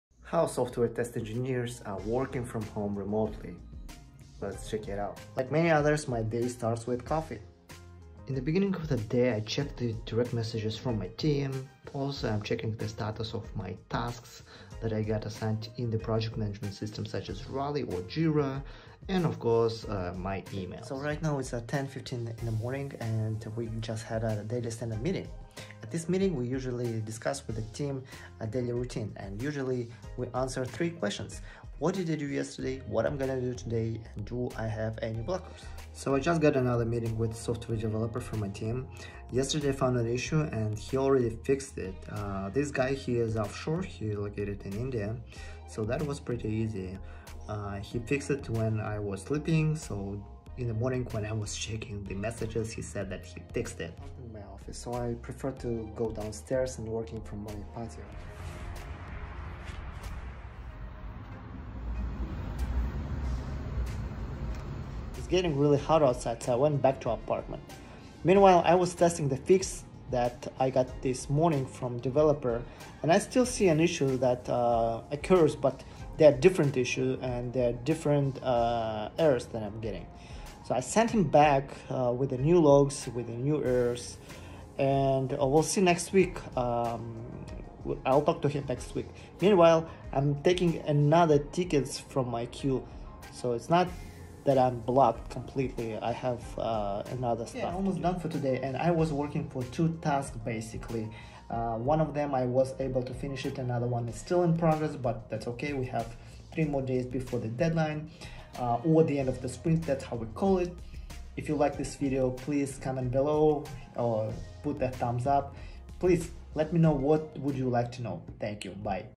A day in the life of Software Engineer in Test. Working from Home
A quick overview of a day in the life of a Software Engineer in Test. This job is rewarding but not easy.

0:12 beginning of the day - checking emails, tickets, jira
0:21 daily stand-up
0:36 meeting with a team member
1:05 testing fix
1:40 wrap up
If you want to become a QA engineer, but are not sure how it looks like?
If you are wondering about software testing interview questions and answers, here is the playlist of the mock interview with hiring managers: